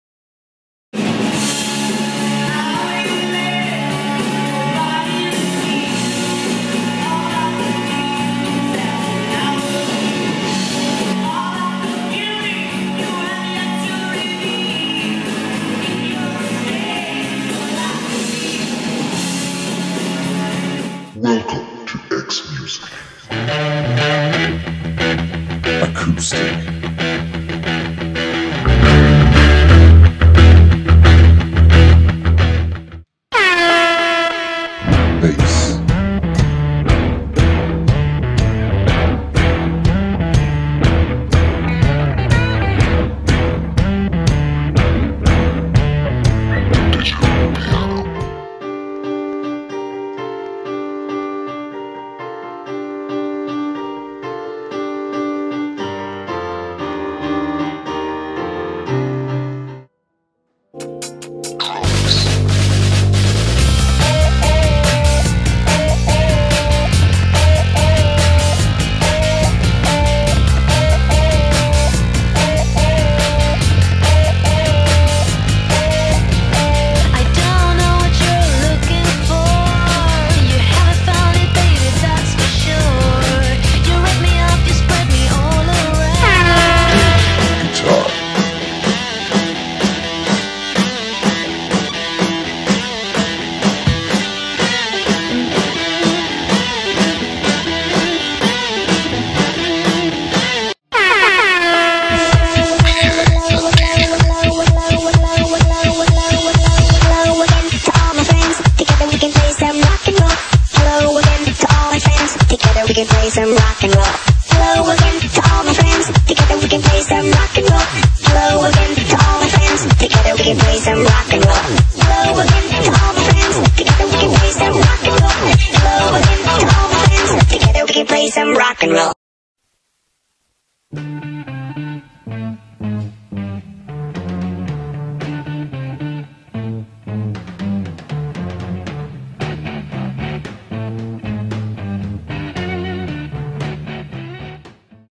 XMusic store video - new
Get a look inside the XMusic Superstore.

(video shot and compiled by student Ciara Kinsella, please support)